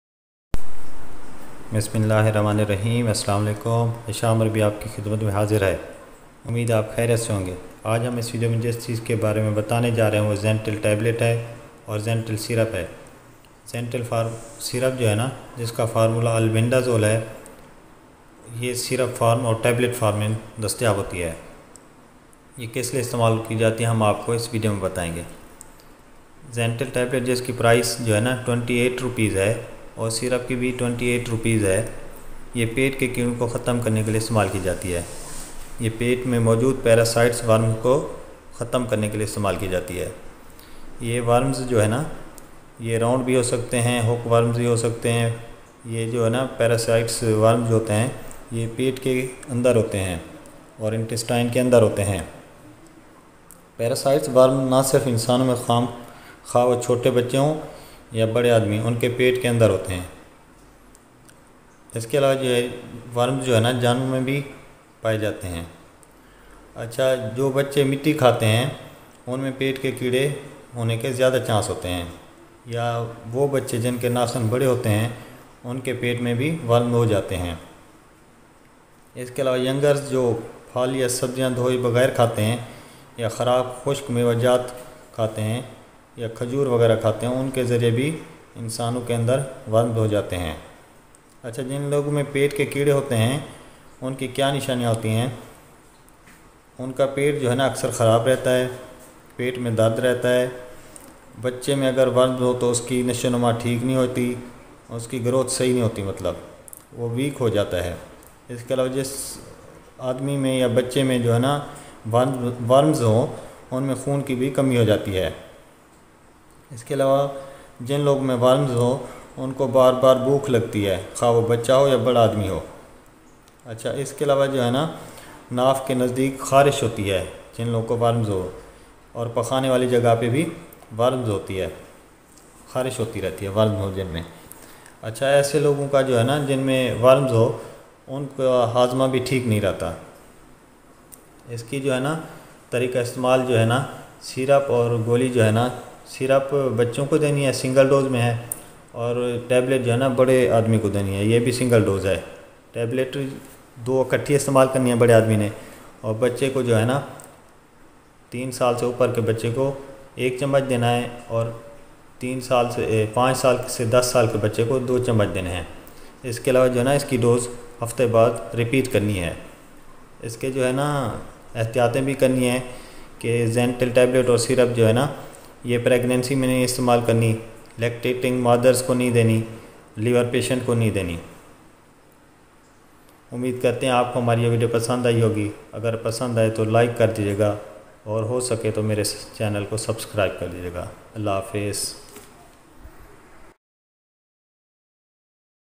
zentel tablet & Syp Uses in Urdu | How To Use zentel tablet in Pregnancy and Lactated, HASHAM ARBI
zentel tablet & Syp Uses in Urdu || How To Use zentel tablet in Pregnancy and Lactated mothers || HASHAM ARBI